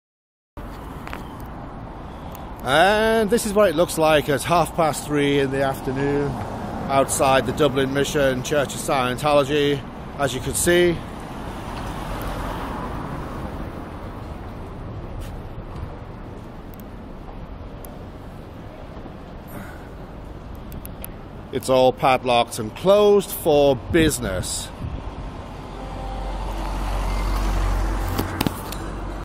Dublin Scientology Mission Padlocked
If you want to "go clear" in Ireland, forget it, nobody cares enough to open the place up.